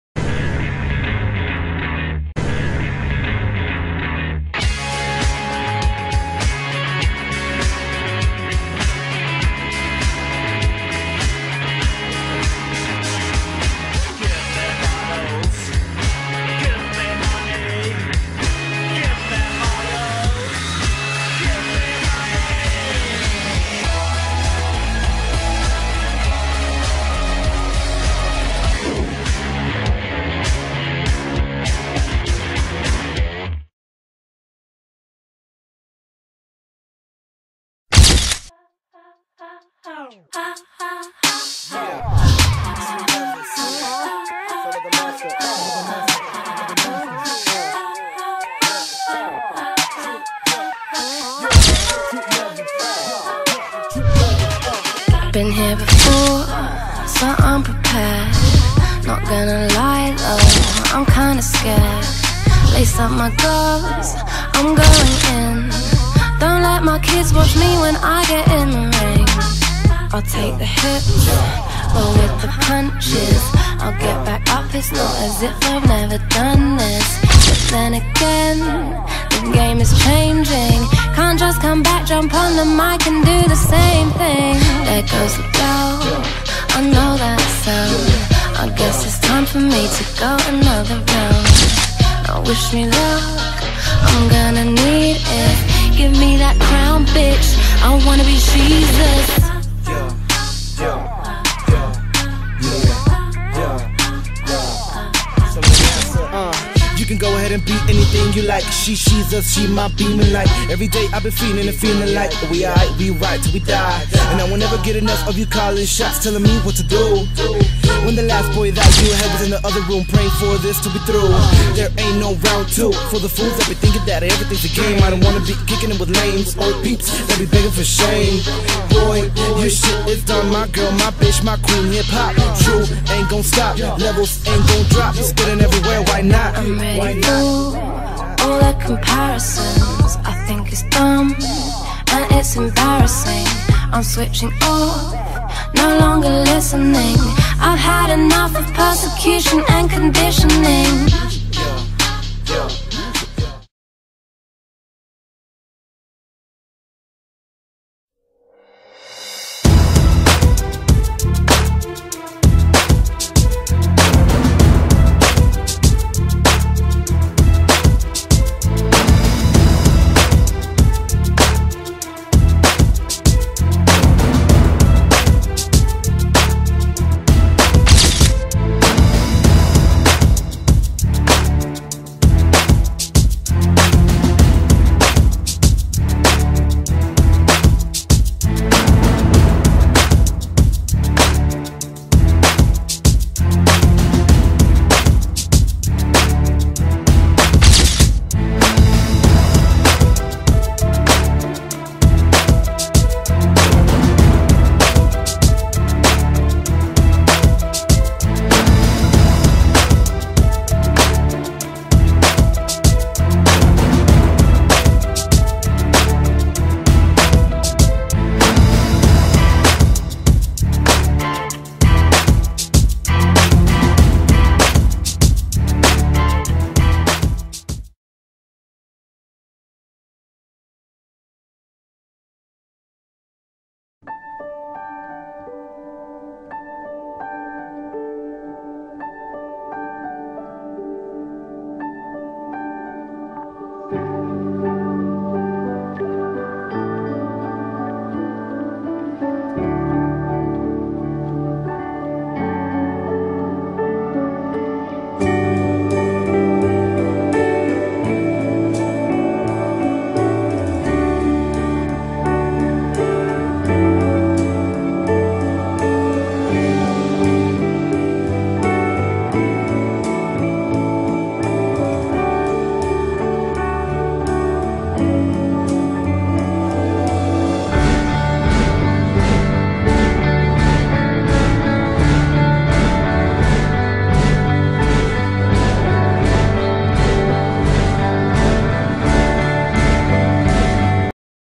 Next SuperModel Cycle 1: Episode 6
*WATCH HD*
SO sorry to eliminated model and her user. It was the toughest decision so far :/
CAST:
Hannah Jones // JustAnNTMFan
Clare Venema // purplepaint143
Molly O'Connell // BankNextTOPModel
Adele Thiel // Luksas2545
5th: Erin Wagner // CenterOfFashion
6th: Analeigh Tipton // S I M L A N A
7th: Jenah Doucette // MaleNTM
8th: Leila Goldkuhl // NextTopModel0505
9th: Raina Hein // CocoNTM